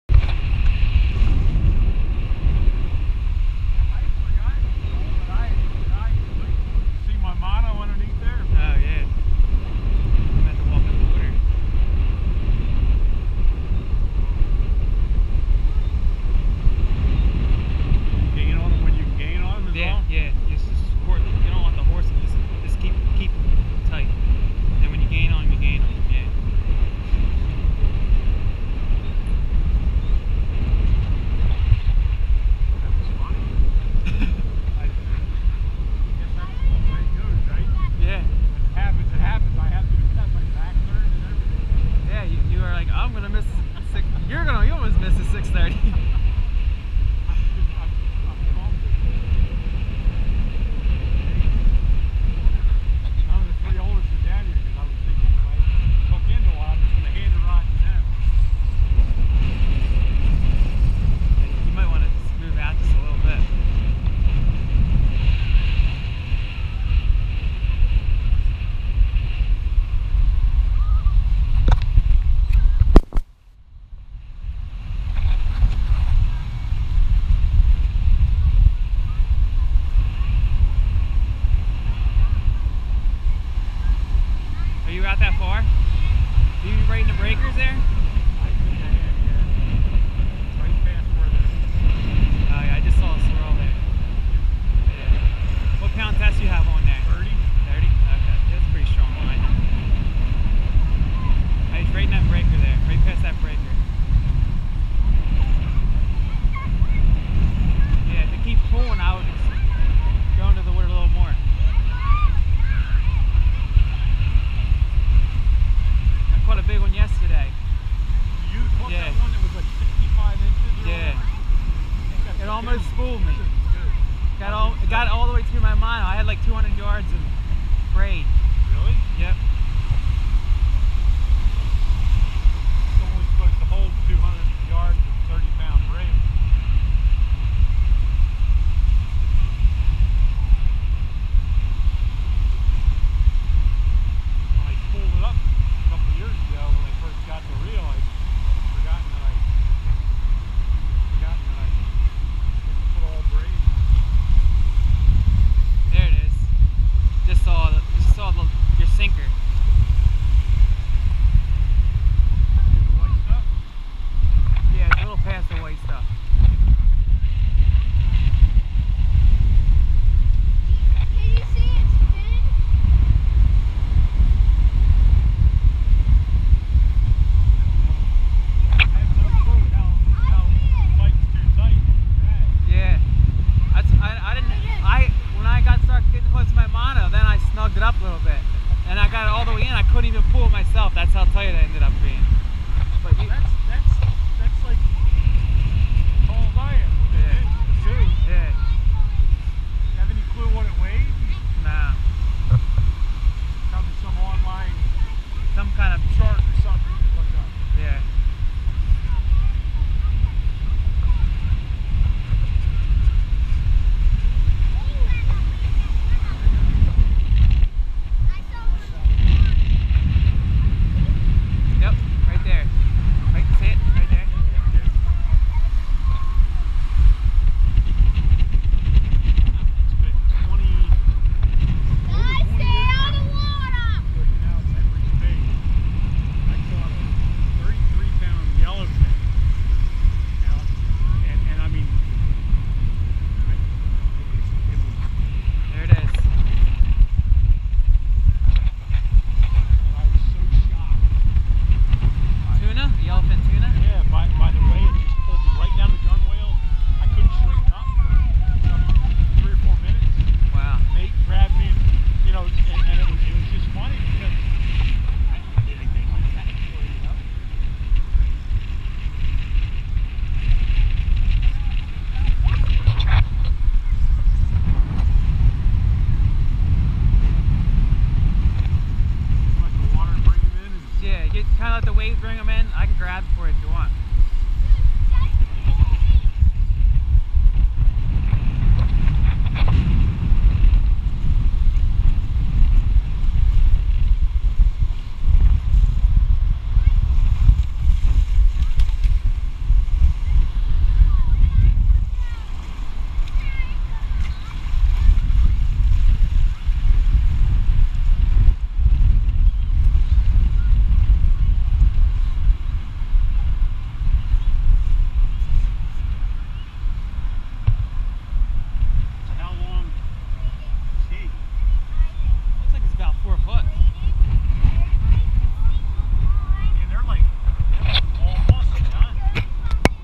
GOPRO Shark fishing OCNJ
My friend catches a nice brown shark.